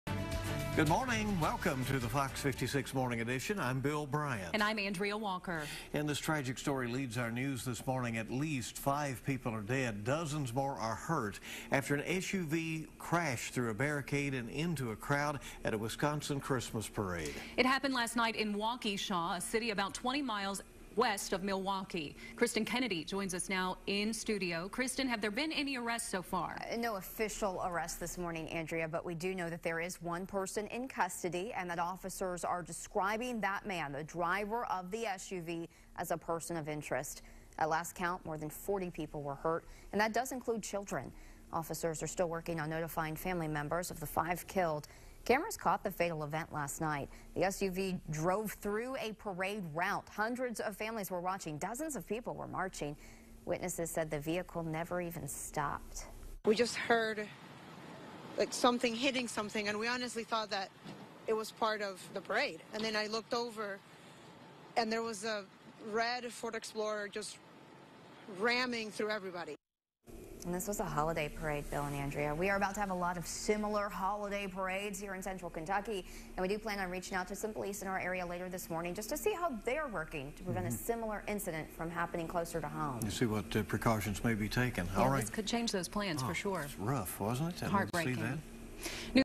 Five dead, 40 injured in crash at Wisconsin Christmas parade
The tragedy happened as an SUV ran into marchers at a Christmas parade in Waukesha, Wisconsin.